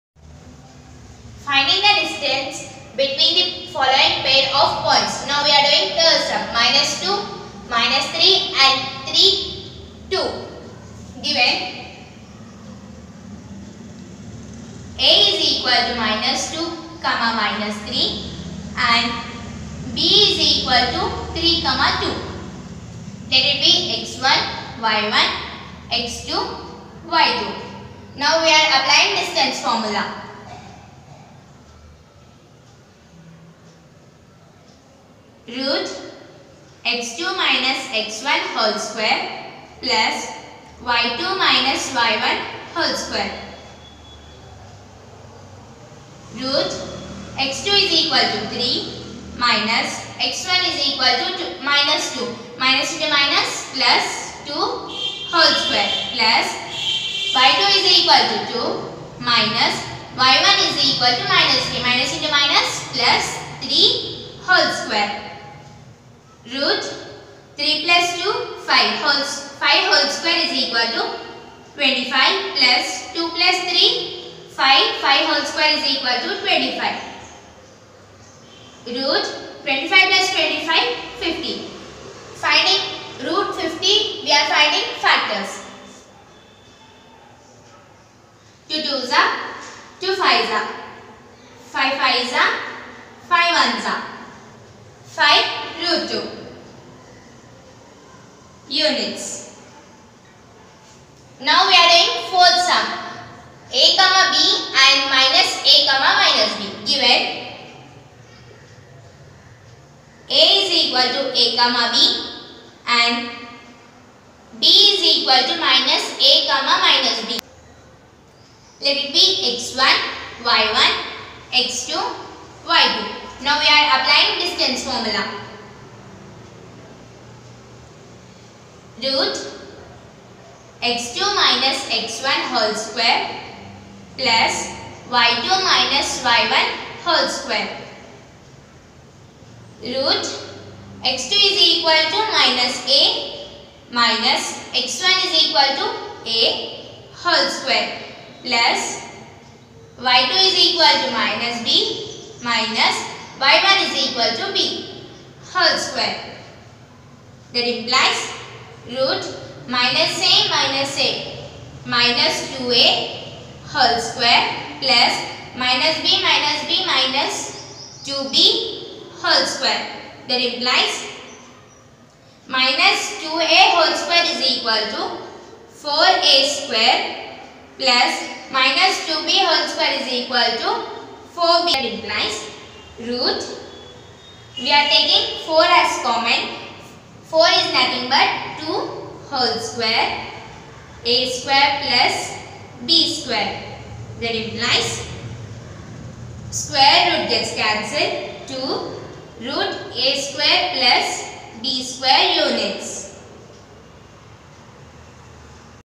Coordinate Geometry / 7.1 sum no.1(iii and iv)
in this video sum no.1(iii and iv), exercise no.1, chapter 7, coordinate geometry of class 10 explained in english by student of st.xaviers high school balanagar